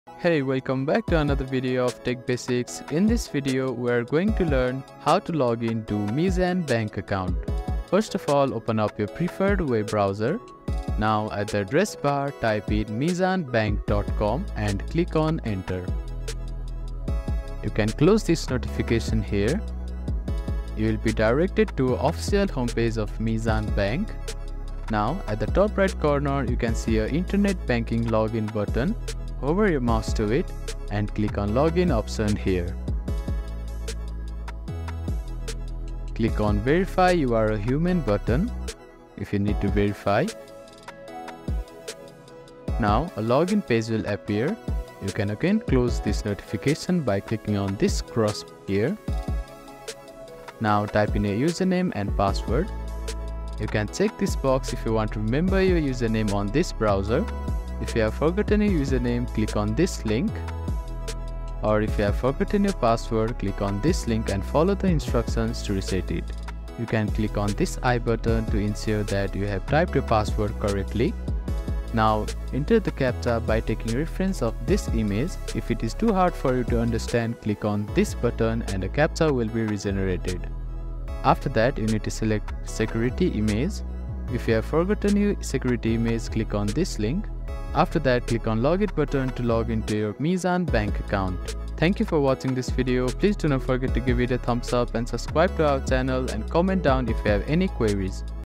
How to Login Meezan Bank Account 2024?
Feeling stumped about logging in to your Meezan Bank account online? Fret no more! This video will be your guide to mastering the login process. We'll take you on a journey from navigating to Meezan Bank's secure login webpage to confidently entering your credentials and accessing your account.
This video is perfect for both new users venturing into online banking and existing customers who might need a quick refresher. We'll break down each step clearly, ensuring you can log in effortlessly and manage your finances conveniently. By the end of this video, you'll be well on your way to becoming a pro at online banking with Meezan Bank.